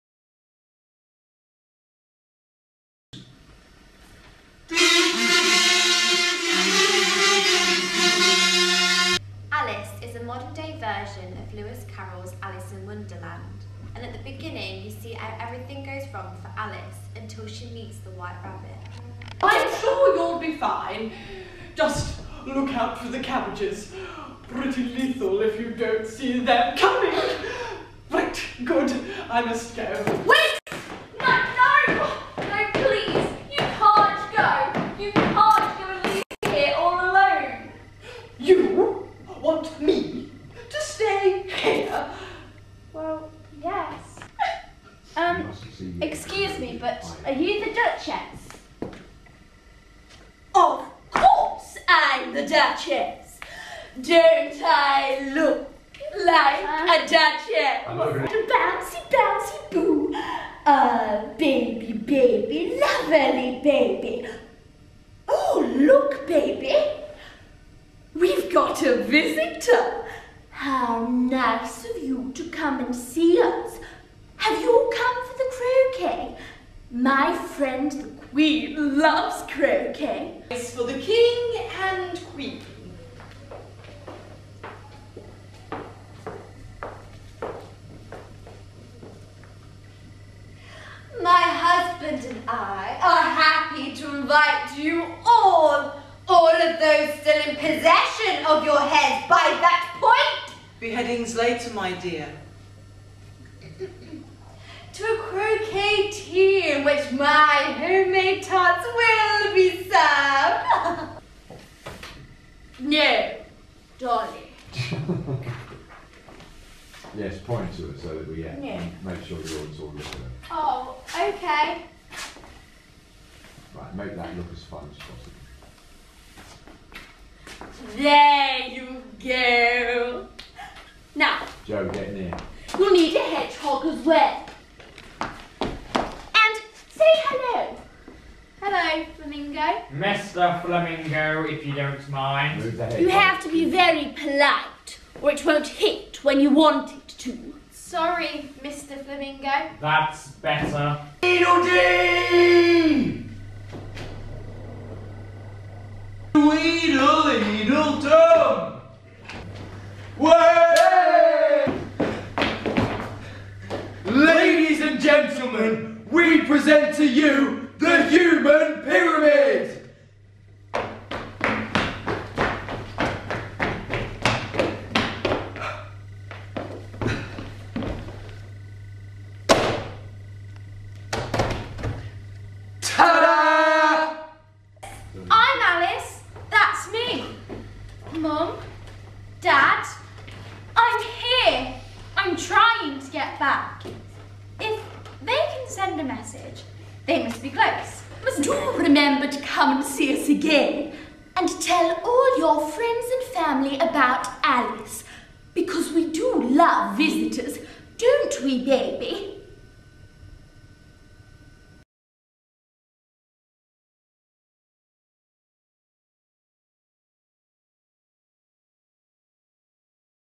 Alice
A modern, humorous look at the novel by Lewis Carroll

The Hatter is…mad. The Dormouse is endearing. The Cheshire Cat won’t stop grinning slyly. The White Rabbit is late for the Duchess…again! A modern look at the adventures of Alice – a girl with a vivid imagination who escapes to find adventures in a very strange Wonderland World. Based on Lewis Carroll’s famous story, you will find talking caterpillars, Wonderland folk, a dormouse and lots more, with comedy and excitement to please all the family and people of all ages.